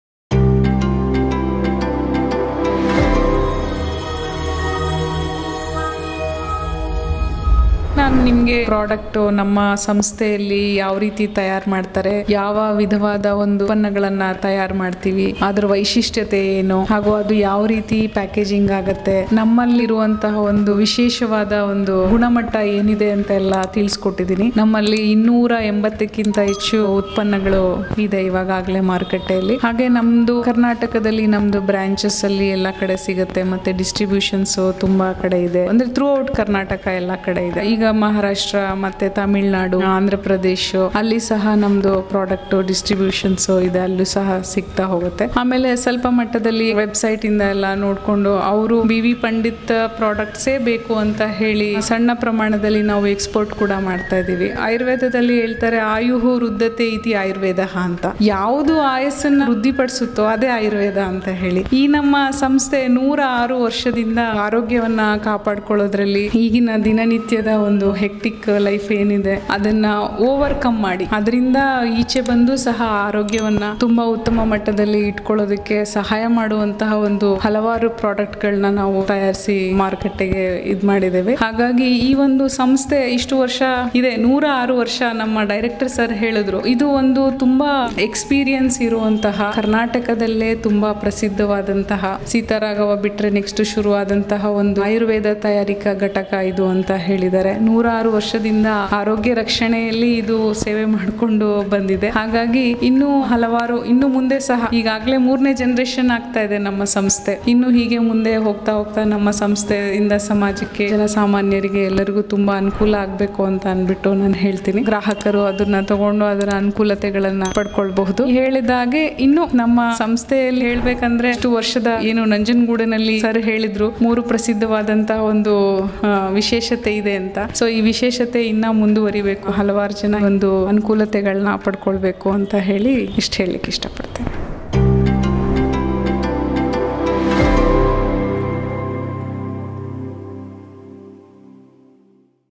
BV Pundit Product Explanation: Dr Geeta M's Conclusion & Thank You to Our Viewers
Thank you for watching the BV Pundit Product Explanation video playlist presented by Dr. Geeta M. In this playlist, Dr. Geeta M provided comprehensive explanations and insights into various products offered by BV Pundit. We hope you found this playlist informative and helpful in understanding the benefits and usage of BV Pundit's products.

We would like to express our gratitude to all the viewers for their support and interest in our videos. Your engagement and feedback are greatly appreciated.

We will continue to bring you valuable content to enhance your well-being. Thank you once again for being a part of our community.